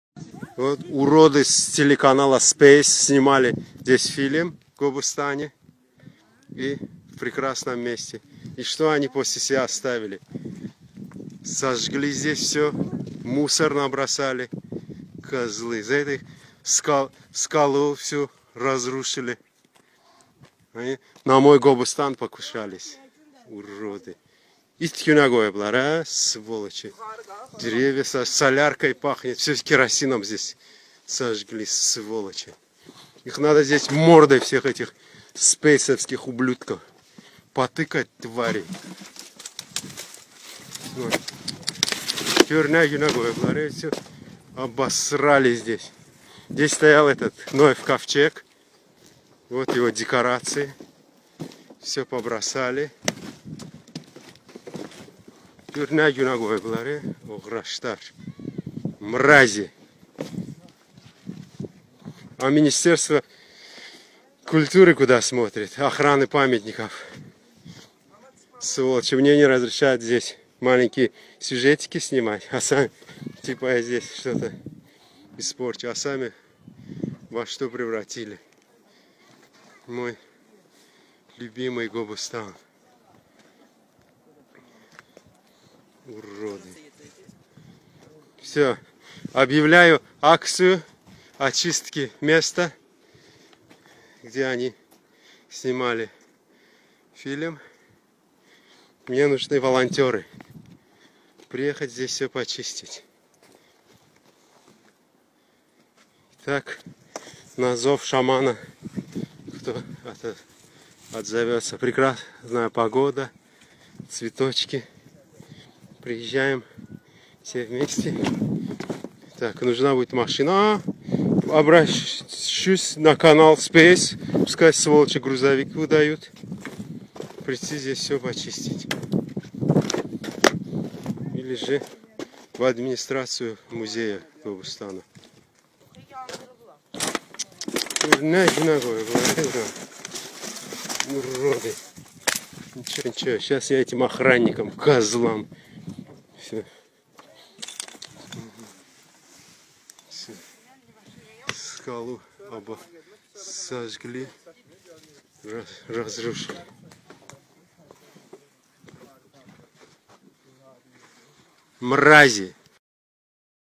Space Tv-nin çəkiliş qrupu Qobustanın tarixi ərazisini acınacaqlı hala saldı.
Qobustan.

Space TV-nin əməkdaşları, çəkiliş qrupu Qobustan qayaları haqda film çəkmək üçün bir neçə gün qoruqda qalıblar. Tarixi ərazini Space TV işçiləri acınacaqlı hala salıblar. Ətraf bütün zibillikdir.